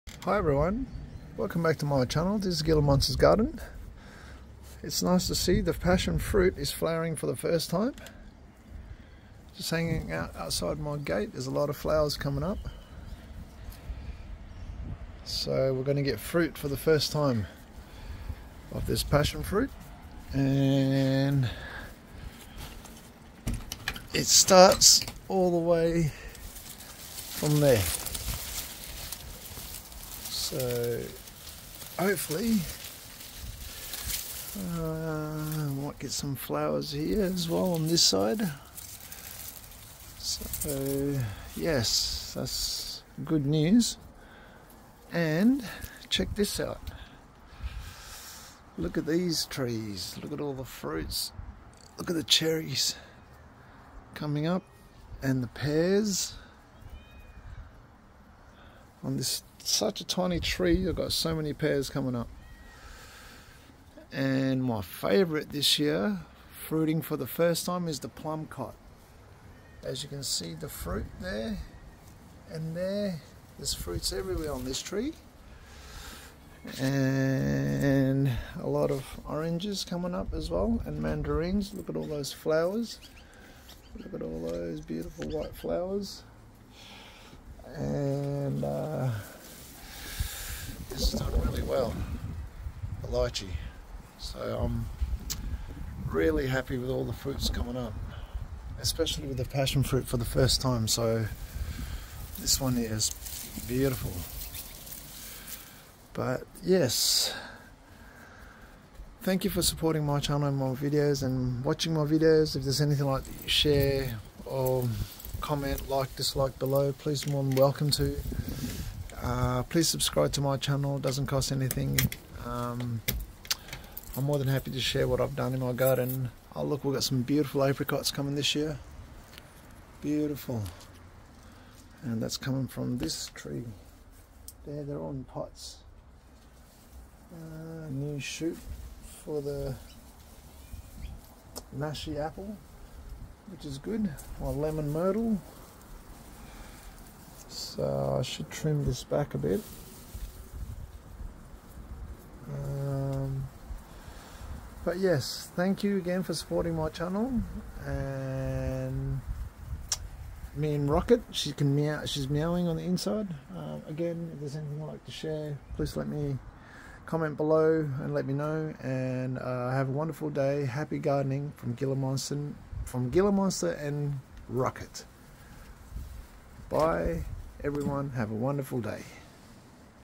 #47 PASSION FRUIT FLOWERING!!OMG!!🤣🤣🍉🍐🍏🍇🍒🍋💯❤️
My passion fruit is flowering for the 1st time, along with  PlumCot,plums crossed with apricot.
Im so happy to see them flowering for the 1st time.
The pears have exploded as well,happy days🤣🤣❤️💯🍋🍐🍉🍊🍍🍉